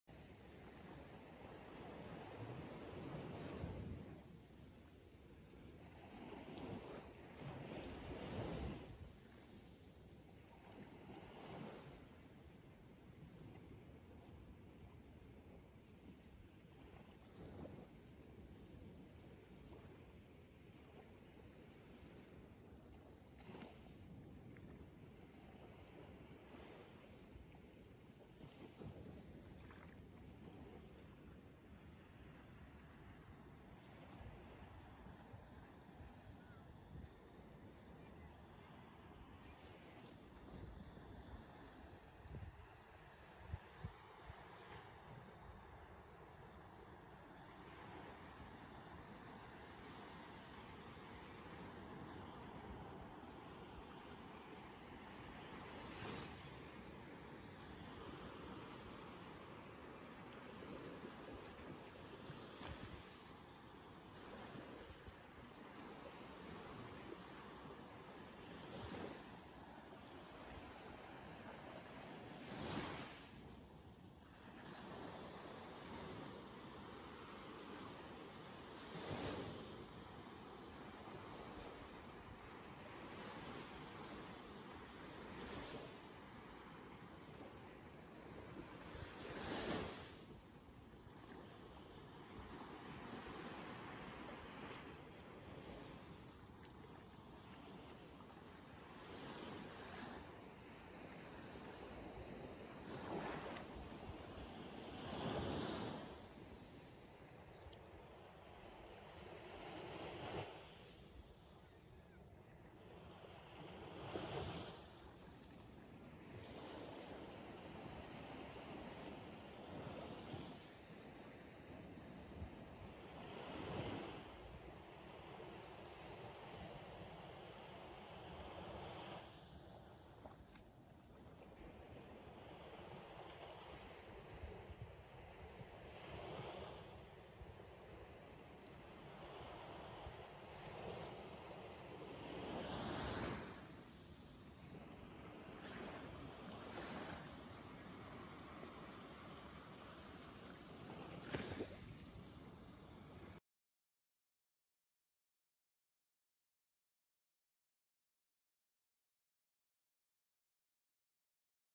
I often release the fish. look to the and
This channel is for those who love fishing, the sea and just relaxing near the water.
I fish with a net. Watch to the end of the video and find out what I got today